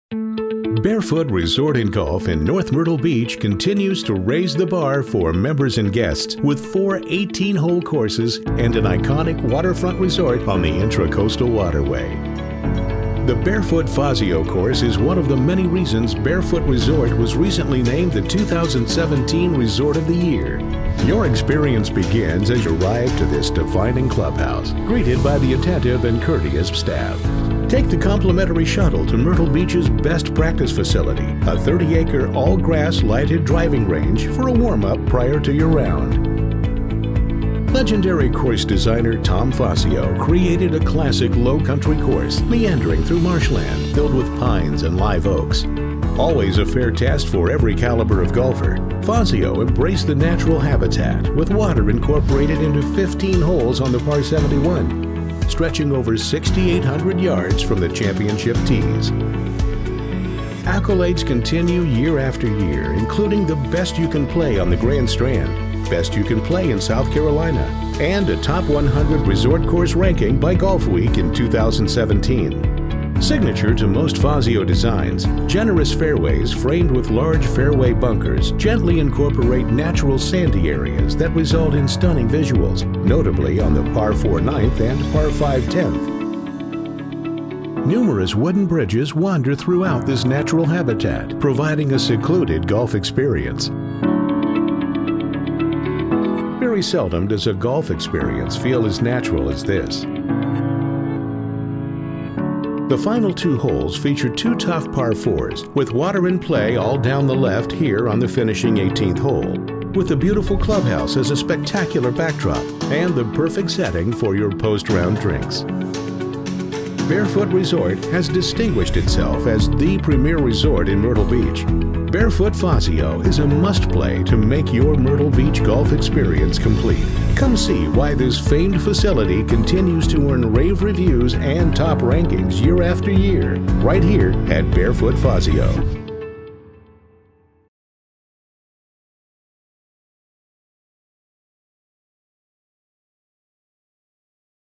A Course From a Legendary Designer | The Fazio Course at Barefoot
Legendary course designer Tom Fazio created a classic Lowcountry course here, meandering through marshland, and filled with pines and live oaks.